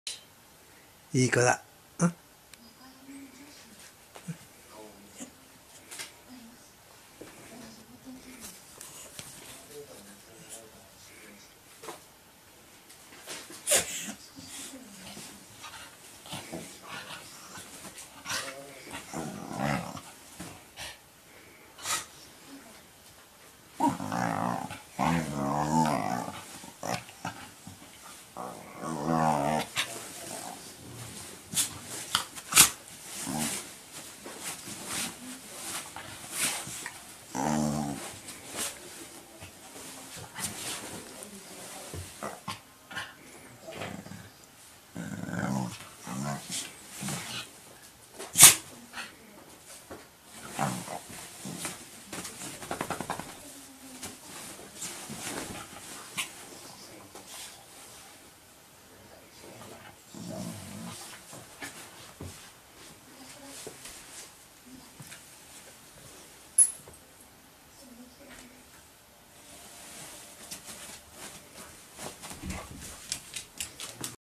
くーちゃん暴れる！2014 4 27
暴れていたくーちゃんはカメラを持って行くと固まってしまいましたが、
しばらくすると、辛抱しきれなくなって暴れ始めました(^O^)
前作'くーちゃん固まる'と、本編、この続きが'欲張りくーちゃん'です。